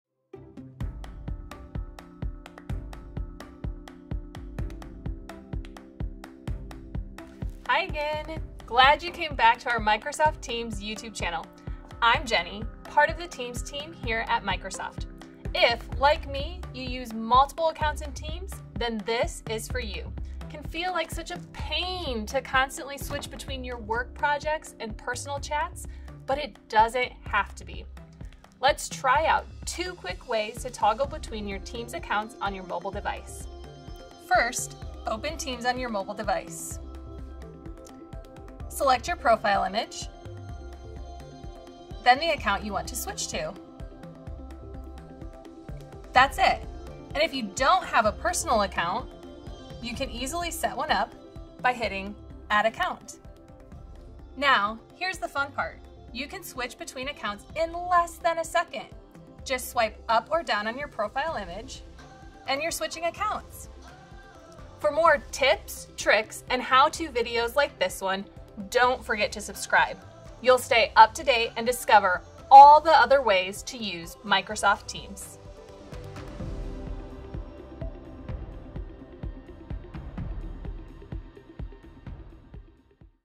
How to Toggle between your Microsoft Teams accounts
Ever wonder how to switch between your accounts when you use more than one in MicrosoftTeams? Jennie Cady, part of the MicrosoftTeams team, shares two quick ways to switch between accounts on your mobile device. Don’t miss out on new tips and all the ways you can use Microsoft Teams!